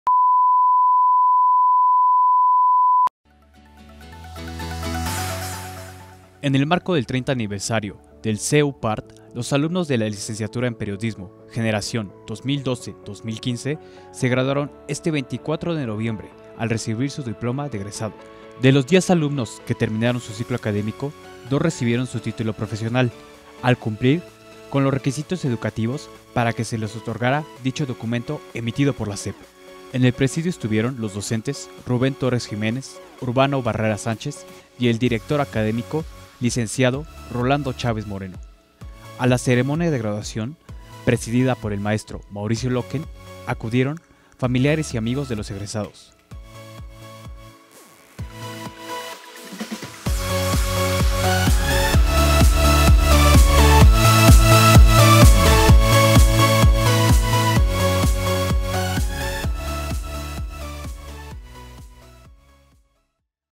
Universidad de Periodismo y Arte, Graduación de Licenciados en Periodismo
Entrega de Diplomas a Licenciados en Periodismo